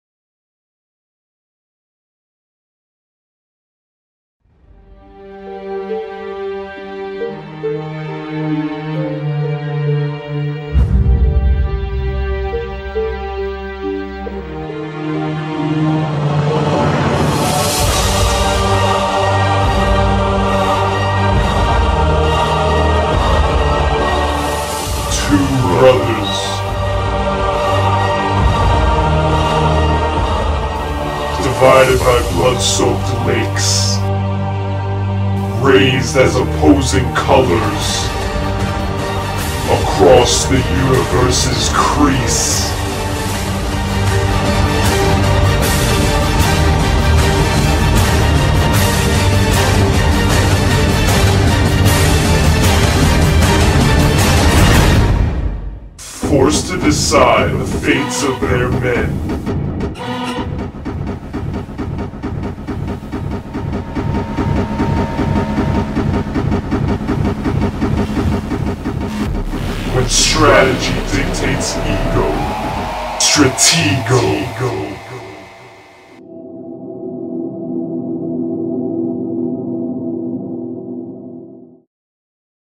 Stratego
The Stratego brothers are at it for the first time, but is it their last?

(edited by sean f & chris h)